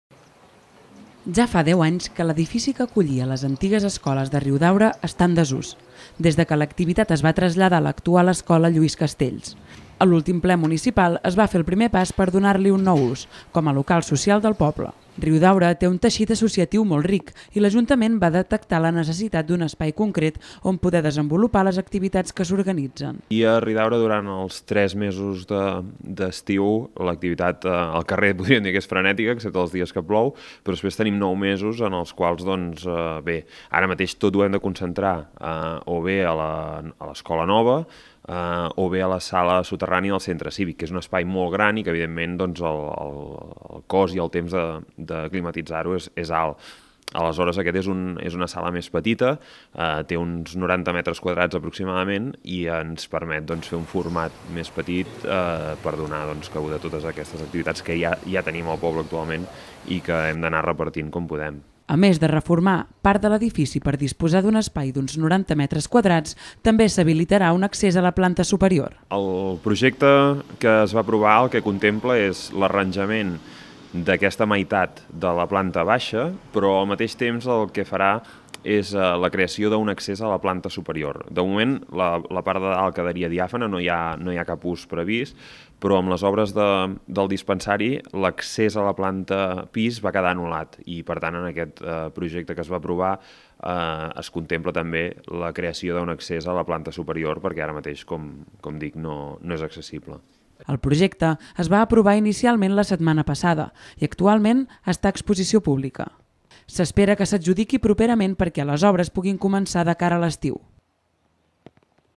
Riudaurà reformarà les antigues escoles perquè siguin el local social del municipi
Riudaura disposarà molt aviat d’un nou local social. Estarà ubicat a l’edifici de les antigues escoles del municipi i busca cobrir la necessitat dels veïns de disposar d’un espai de trobada on fer-hi activitats.  L’Ajuntament ha aprovat inicialment el projecte, valorat en 94 mil euros.